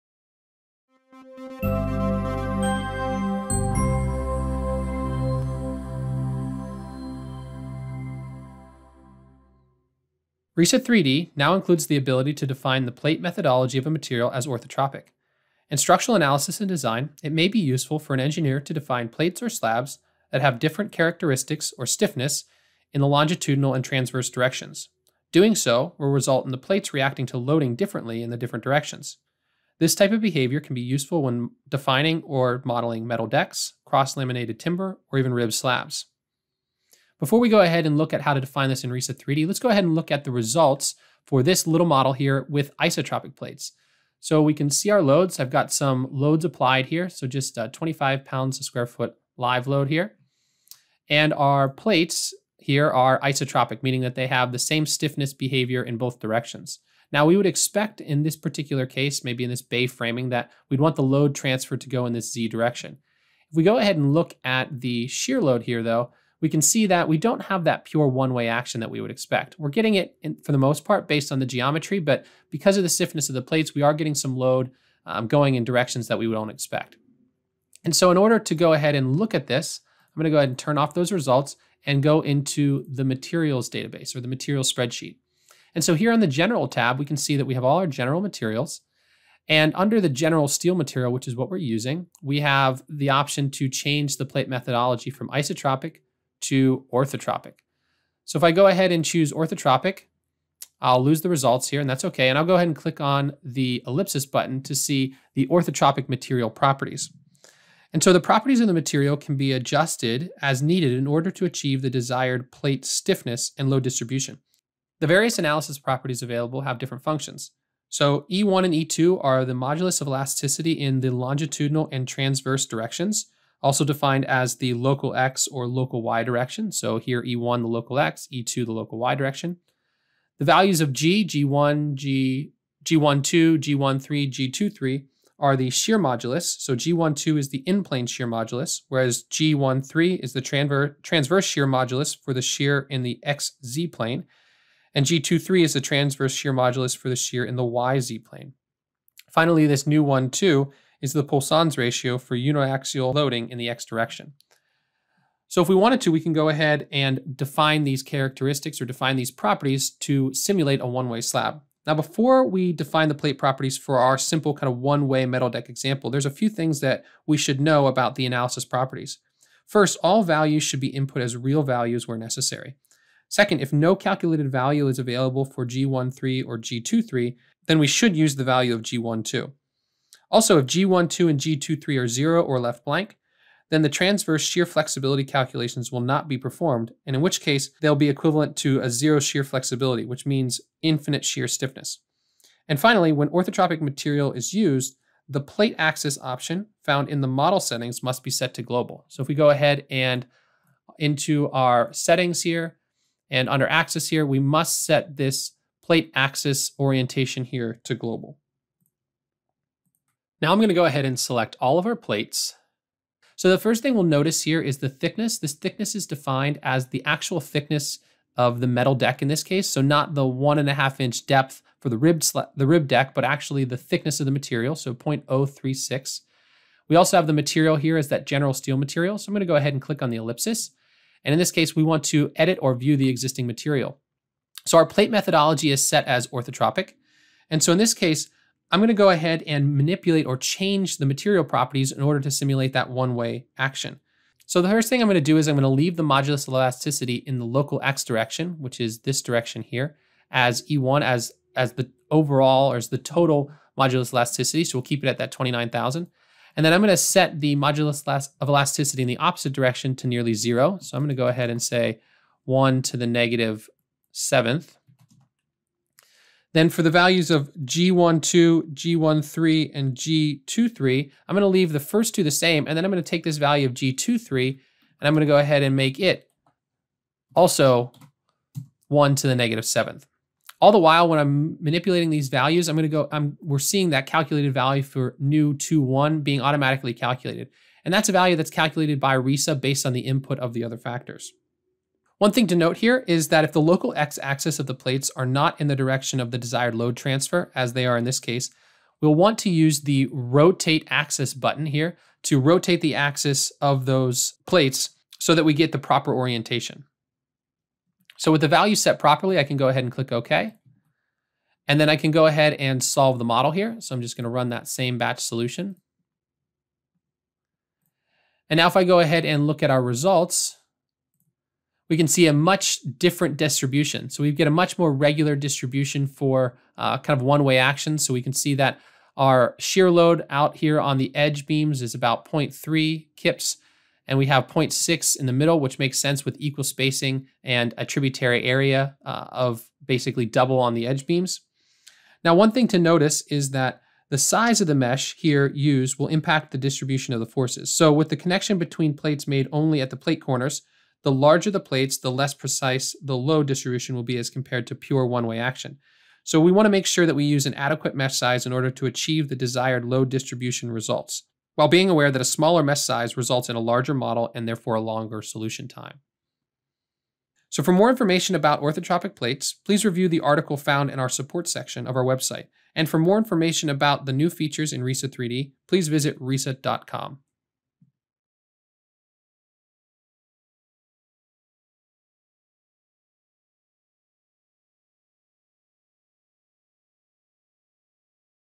Orthotropic Plates in RISA-3D
RISA-3D now includes the ability to define the plate methodology of a material as orthotropic, giving users the ability to easily define and analyze metal decks, cross-laminated timber or ribbed slabs.